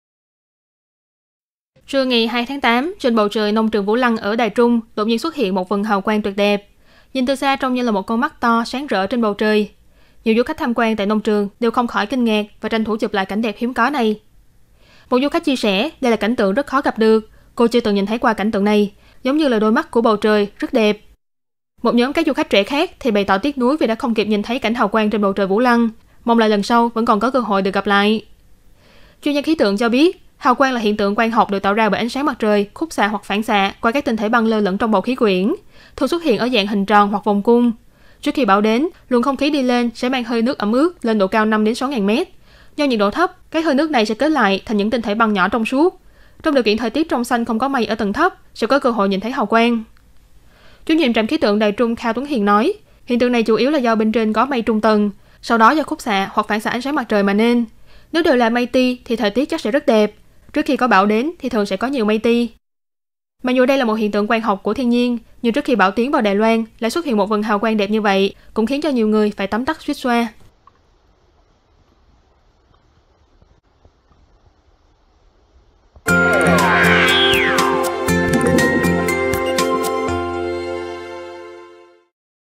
Trước ngày bão Khanun, bầu trời Nông trường Vũ Lăng xuất hiện vầng hào quang tuyệt đẹp
Trưa ngày 02/08, trên bầu trời Nông trường Vũ Lăng ở Đài Trung đột nhiên xuất hiện một vầng hào quang tuyệt đẹp, nhìn từ xa trông như là một con mắt to, sáng rỡ trên bầu trời, nhiều du khách tham quan tại nông trường đều không khỏi kinh ngạc và tranh thủ chụp lại cảnh đẹp hiếm có này.

Đài Phát thanh Quốc tế Đài Loan (Radio Taiwan International)
Ban Việt Ngữ
Lịch phát sóng chương trình
Inbox: m.me/banvietnguRTITaiwan